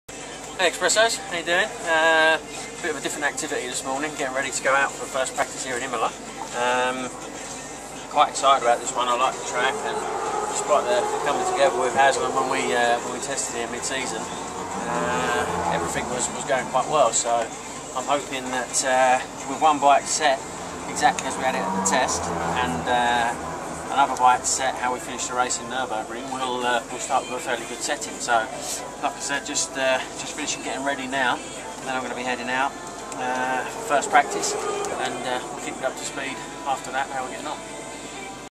Shakey - Looking ahead to Imola
Shakey takes time out to chat to his fans, and give us a little insight to his thoughts ahead of free practice at Imola.